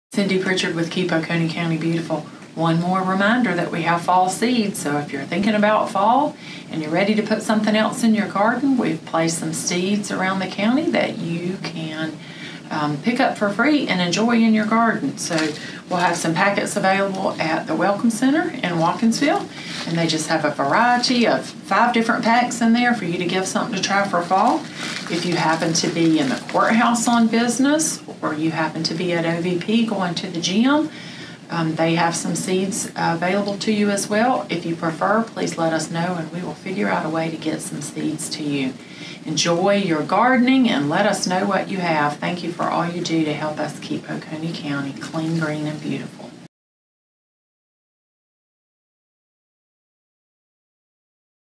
KOCBC Has Fall Seeds to Share
Keep Oconee County Beautiful wants to share fall seeds with you! for more information about the Keep Oconee County Beautiful Commission and its programs and activities.